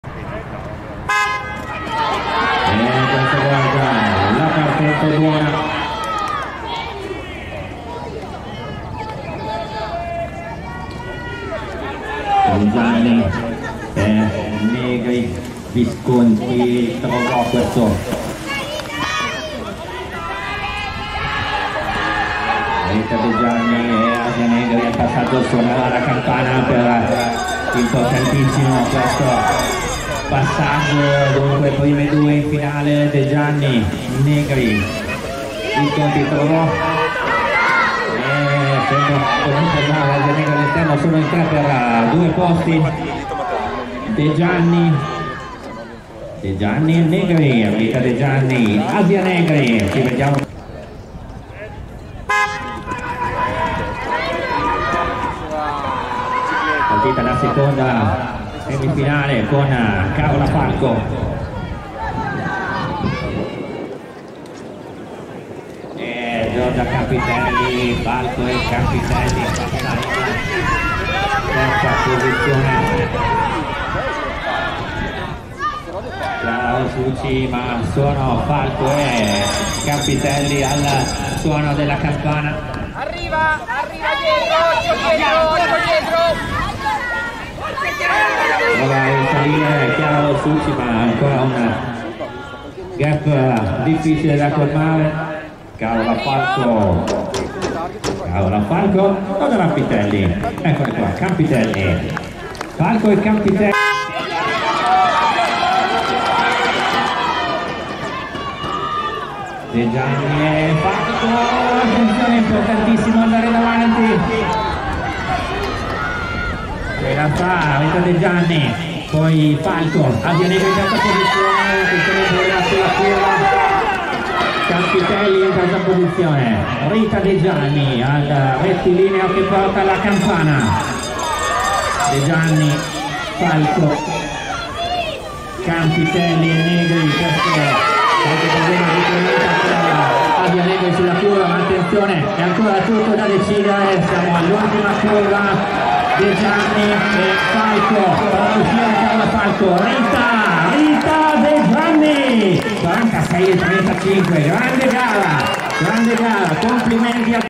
Trofeo "Vesmaco" Senigallia 2022 - A/F 500m sprint, semifinali e finale
Pattinaggio velocità su rotelle, specialità pista (speed skating on wheels, track)
Trofeo "Vesamco" Senigallia 2022
15 Maggio 2022, Senigallia (AN) - Italy
Categoria Allievi Femminile
500m sprint, semifinali e finale

Semifinale, Risultati batteria n.1
1 265 DE GIANNI RITA 302 S.S.D.S.MENS SANA 45.990
2 204 NEGRI ASIA 3614 A.S.D.SPORT INSIDE 46.090
3 50 VISCONTI DALIA 1091 C.P.G.A. ASD 46.260
4 477 TROVO' EMMA 98 SKATING C.ROVIGO 47.490

Semifinale, Risultati batteria n.2
1 36 FALCO CAROLA 3809 A.S.D.BRIANZA INLINE 47.110
2 29 CAMPITELLI GIORGIA 1260 ROLLING BOSICA 47.250
3 45 ROSUCCI CHIARA 302 S.S.D.S.MENS SANA 47.710
4 26 CLEMENTONI SOFIA 1260 ROLLING BOSICA 49.330

Finale Risultati
1 265 DE GIANNI RITA 302 S.S.D.S.MENS SANA 46.360
2 36 FALCO CAROLA 3809 A.S.D.BRIANZA INLINE 46.410
3 204 NEGRI ASIA 3614 A.S.D.SPORT INSIDE 46.570
4 29 CAMPITELLI GIORGIA 1260 ROLLING BOSICA 46.610